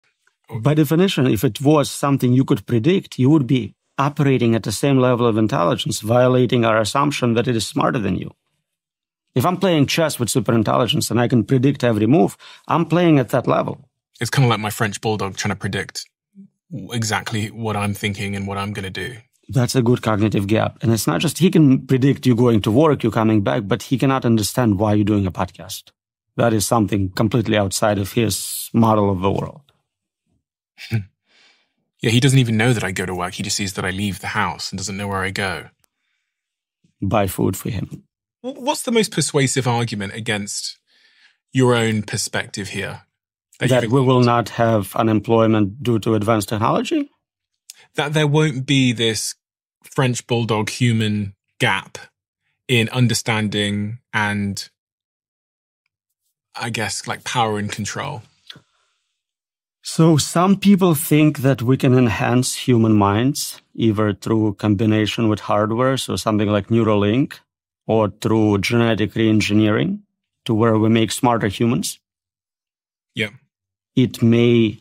Unleashing the French Bulldog of AI: What Are They Really Thinking?
Ever wondered why predicting the behavior of AI feels like trying to understand a French bulldog's thoughts? Dr. Roman Yampolskiy dives deep into the cognitive gap between human intelligence and superintelligent AI. Explore the potential implications of advanced technology on jobs and the very fabric of society. Discover whether enhancement through hardware or genetic re-engineering could bridge the divide or lead us to unforeseen consequences. Get ready for a thought-provoking discussion on the future of humanity and AI's staggering potential!